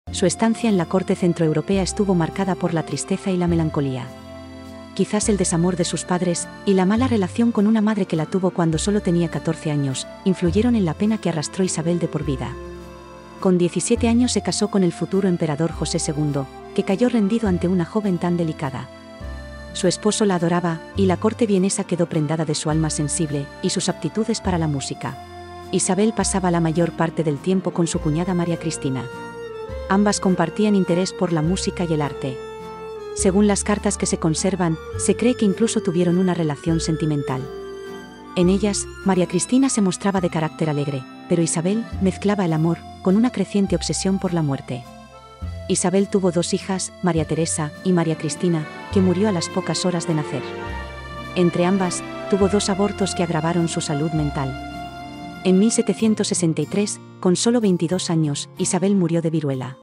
La princesa triste. Isabel de Borbón-Parma
Isabel María Luisa Antonieta Fernanda Josefa Javiera Doménica Juana de Borbón-Parma nació el 31 de diciembre de 1741 en el Palacio del Buen Retiro, en Madrid. Fue la primera hija de Felipe, infante de España y futuro duque de Parma, y de su esposa, la princesa Luisa Isabel de Francia, hija mayor del rey Luis XV de Francia.